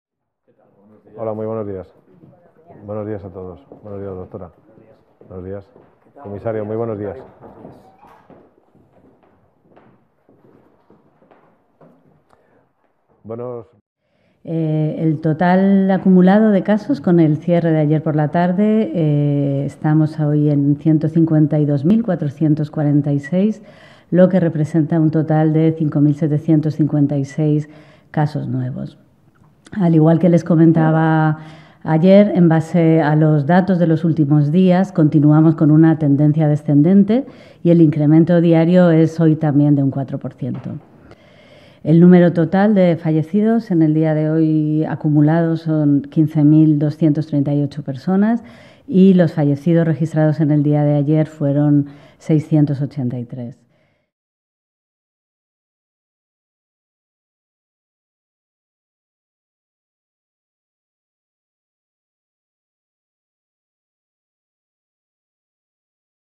Spain sees daily drop in virus deaths, over 15,000 total
Spain's Health Ministry announces the daily death toll from the coronavirus has fallen to 683, after rising for two days. The country, one of the worst-hit by the pandemic, has now recorded 15,238 deaths from COVID-19. Video by PALACIO DE MONCLOA via AGENCE FRANCE-PRESSE (KGA)